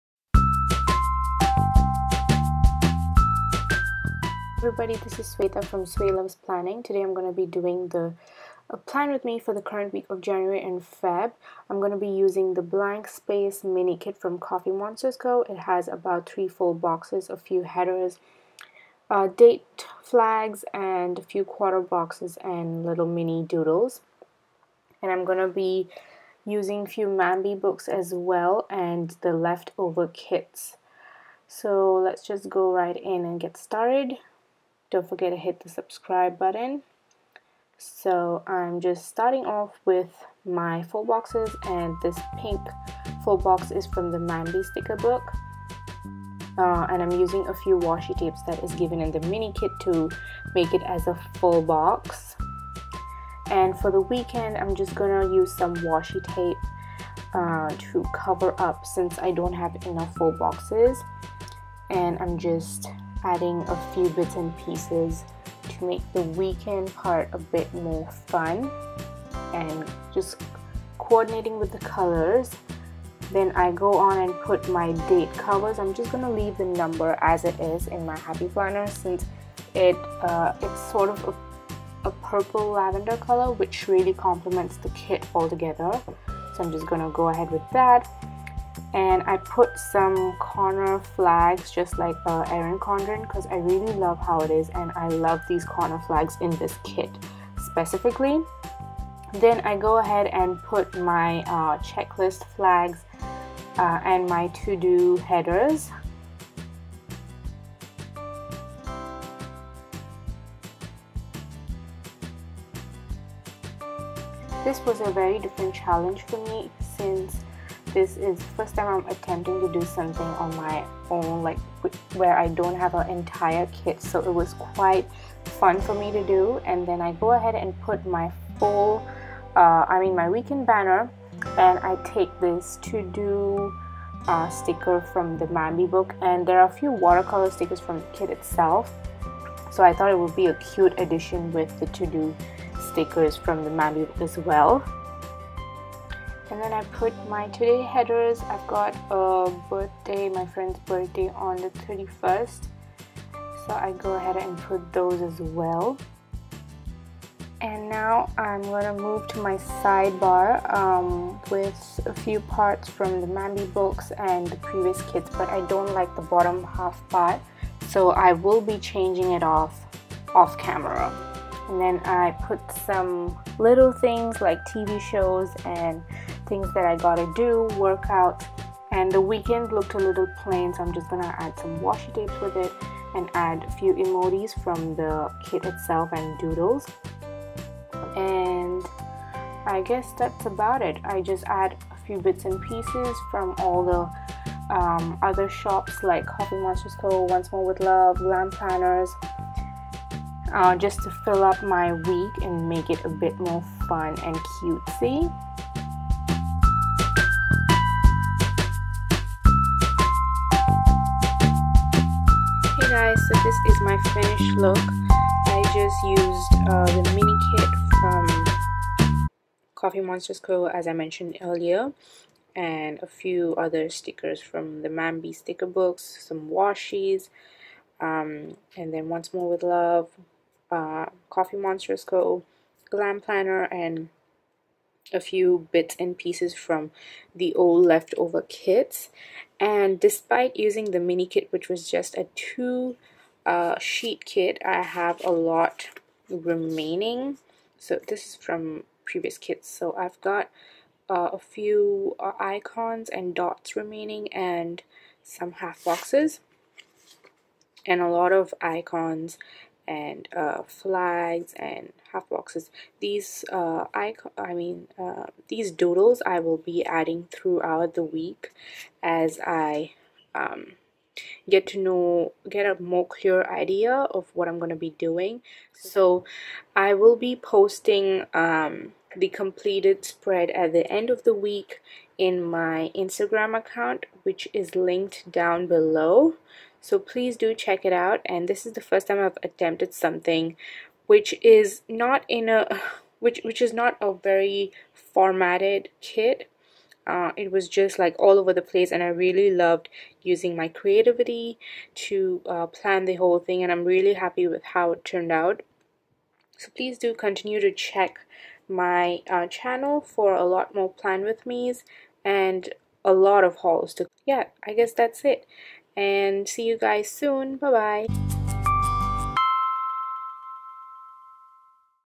PWM: Blank Space Mini kit: TCMC
Plan with Me in my Happy Planner using the The Coffee Monsterz Co: Blank Space mini kit!

\\Kit//

\\Shops mentioned//
The Coffee Monsterz Co
Once more With Love
Glam Planner

Things used:
Friskars Crafts knife
Recollections Washi tube
Daiso Deco pens

All feedback and opinions are my own!